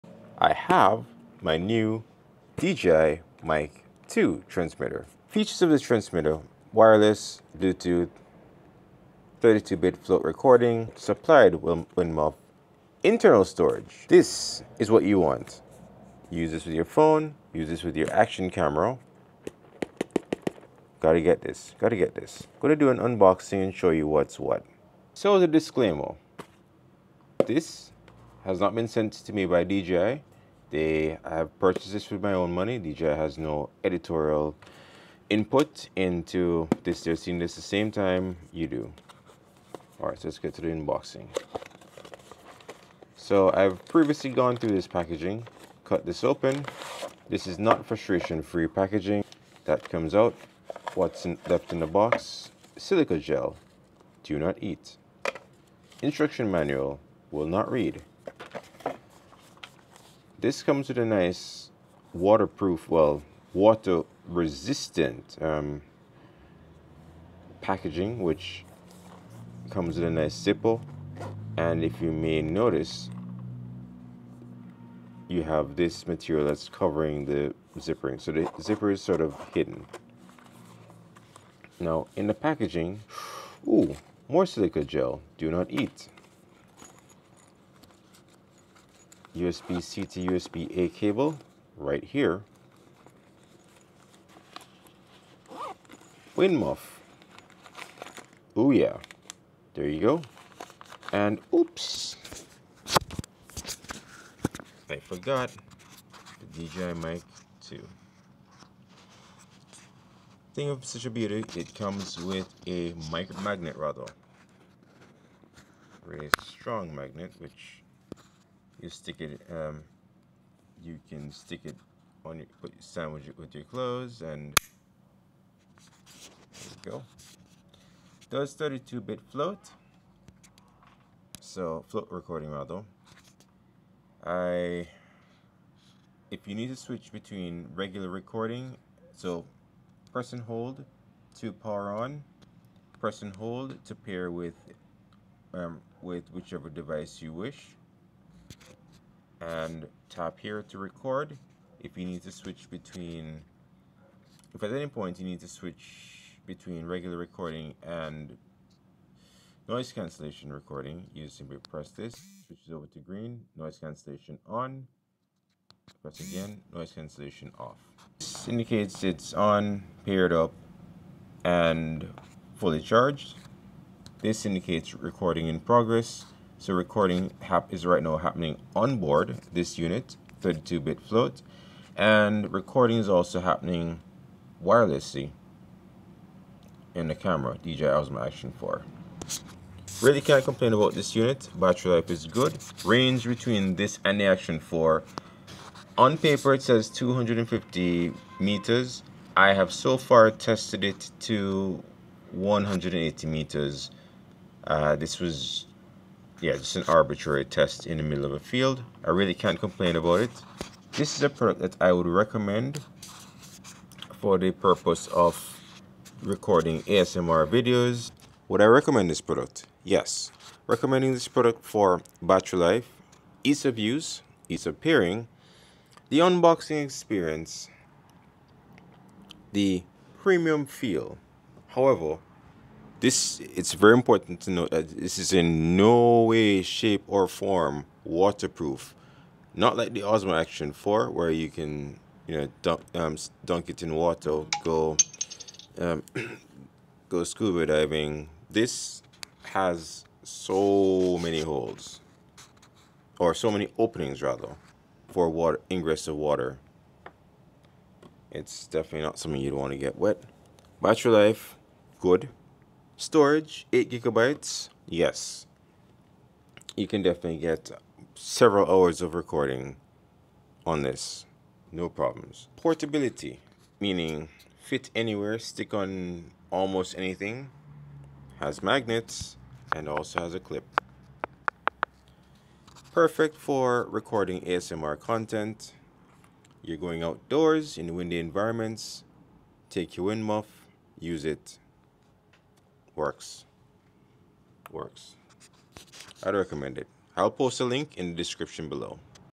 DJI Mic2 TX unboxing
This is a quick unboxing and product review of the DJI Mic2 as paired with my DJO Osmo Action 4. Note there is a clerical error with the narrated unit used. The 180 meters stated was to be 180 feet which works out to about 55 of the 70 meters.